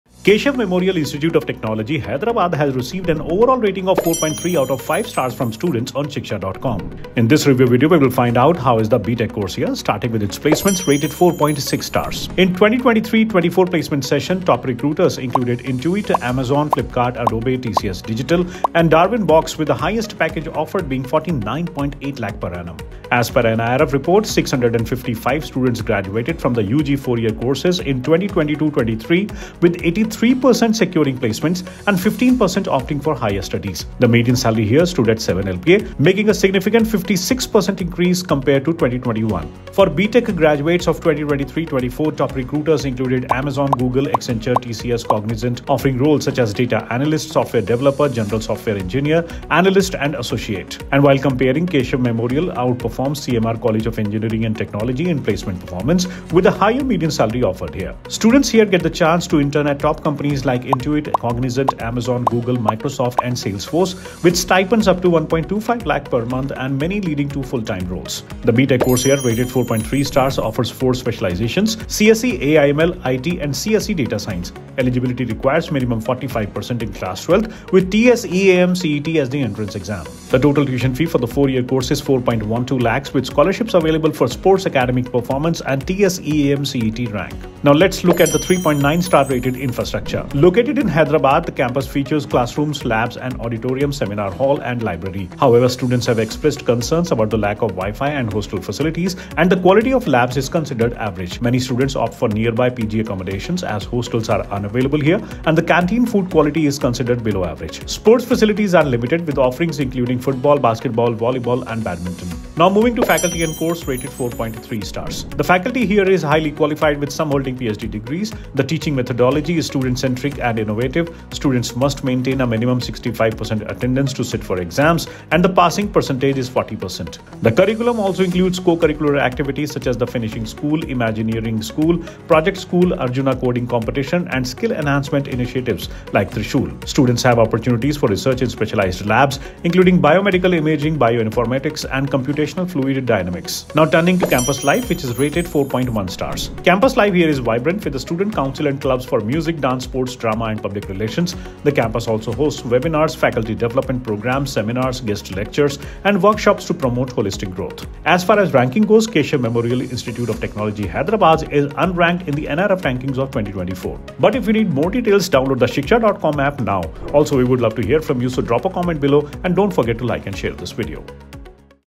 KMIT Hyderabad BTech Review
Discover our detailed review of the BTech program at Keshav Memorial Institute of Technology. Learn about the curriculum, faculty expertise, campus facilities, placement opportunities, and student experiences. Watch this video to find out if Keshav Memorial Institute of Technology is the right choice for your BTech studies.

0:03 - Overall Rating
0:12 - Placements
1:10 - BTech Specialisations
1:20 - Fees
1:29 - Infrastructure
1:53 - Faculty
2:25 - Campus Life